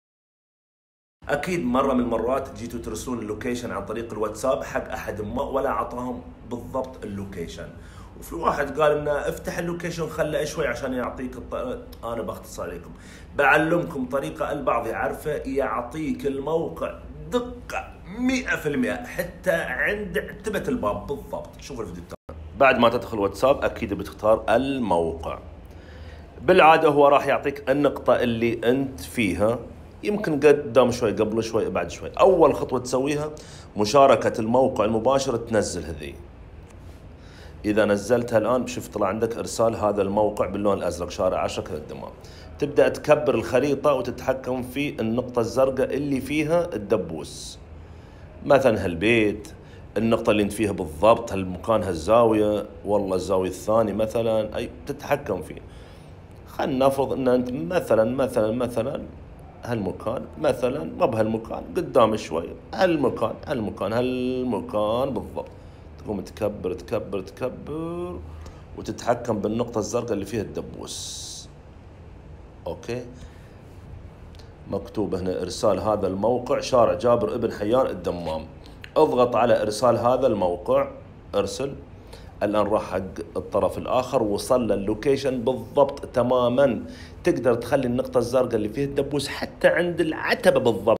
شرح ارسال اللوكيشن ب الوتساب بدقة 100٪ لجهاز الايفون
هل تواجه مشكلة عدم دقة موقعك (اللوكيشن ) لأرساله من خلال تطبيق الواتس اب ؟ هذه طريقة سهلة لأرسال اللوكيشن بدقة 100٪